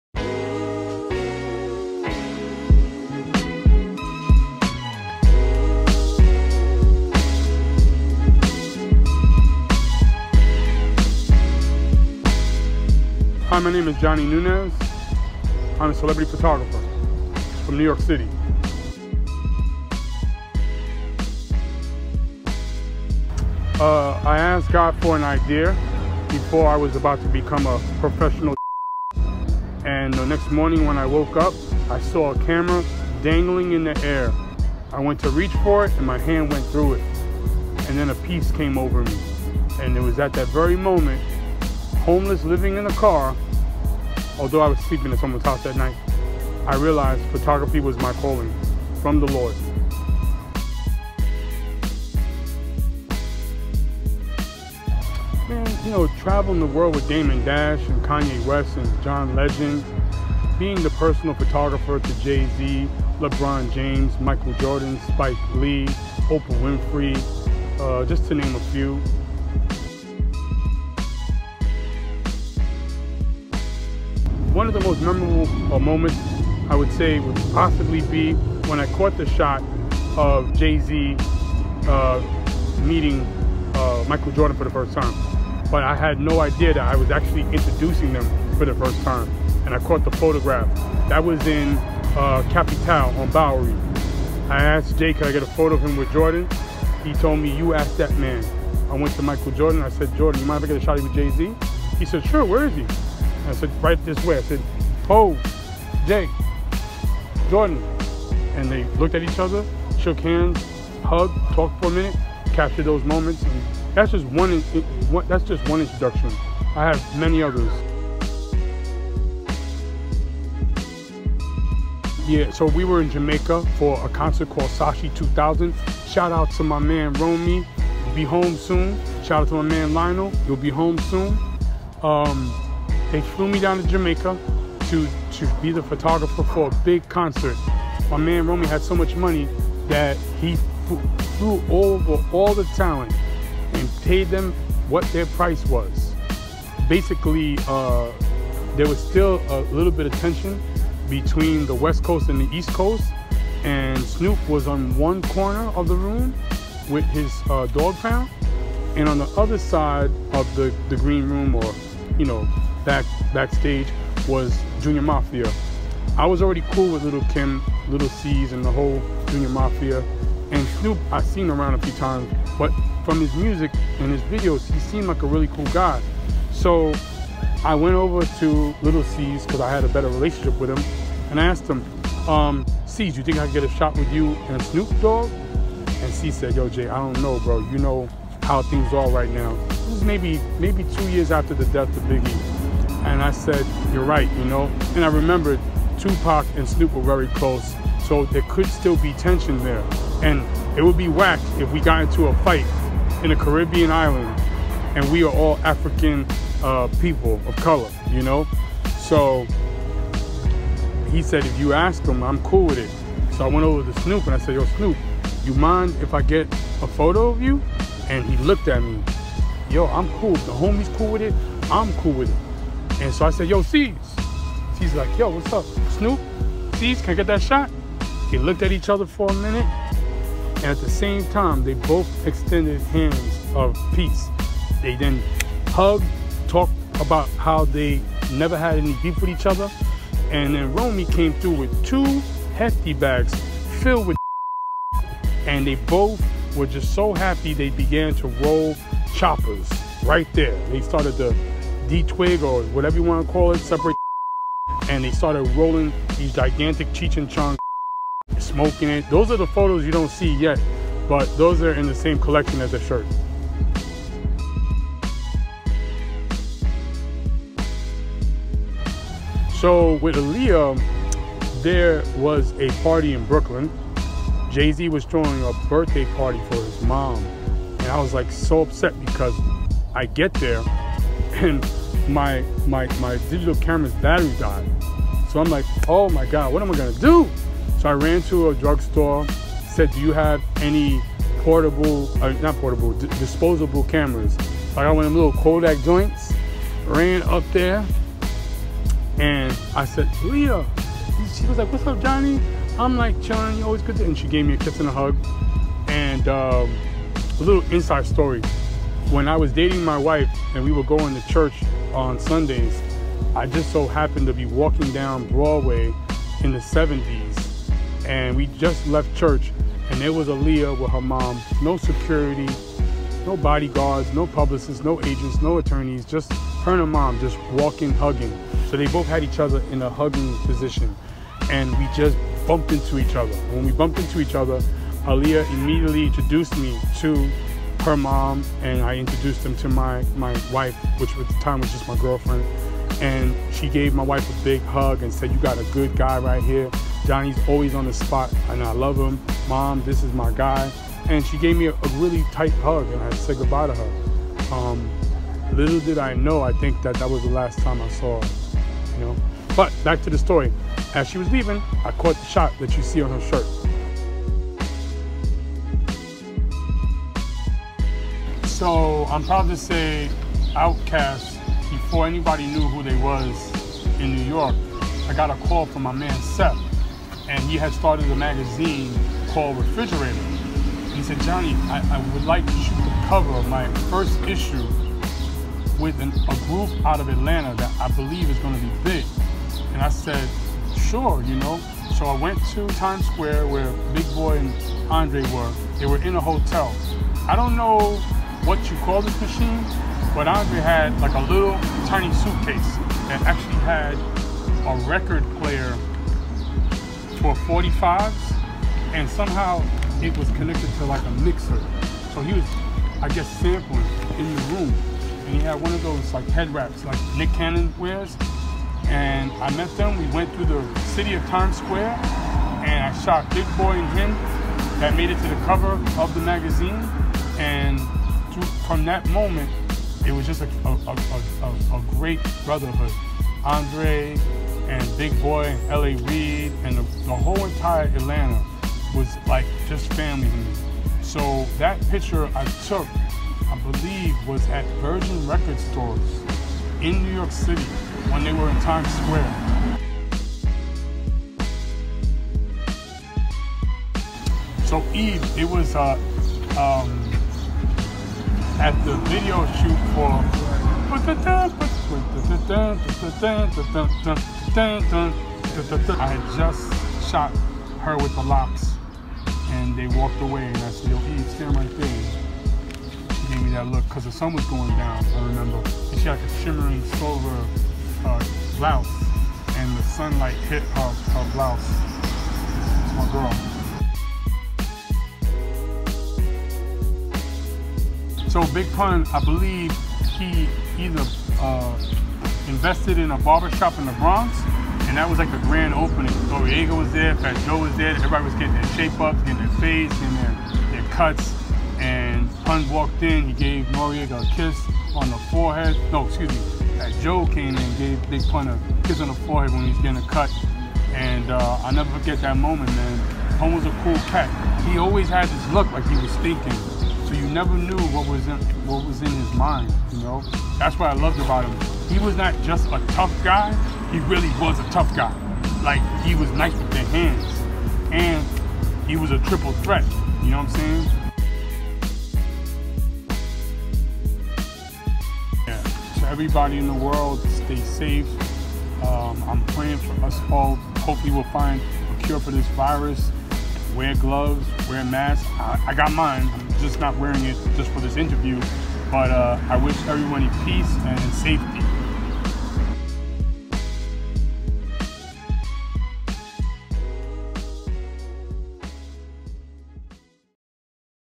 A Conversation With Johnny Nunez - Lafayette S/S 2020 Collection
Lafayette x Johnny Nunez Capsule Collection